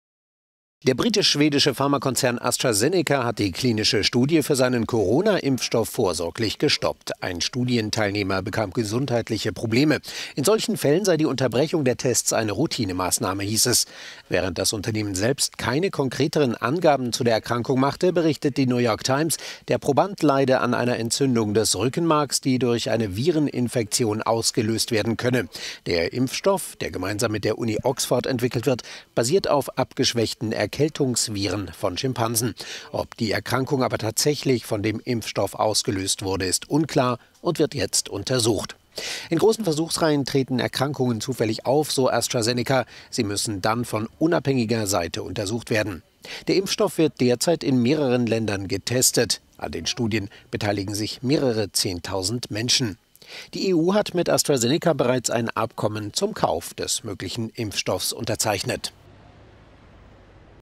"Schwere Nebenwirkung": AstraZeneca setzt Tests an Corona-Impfstoff aus
Der britisch-schwedische Pharmakonzern hat die Tests am mit der Uni Oxford entwickelten Impfstoff gegen das Coronavirus zunächst gestoppt, weil Probanden unter "schwerwiegenen Nebenwirkungen" litten.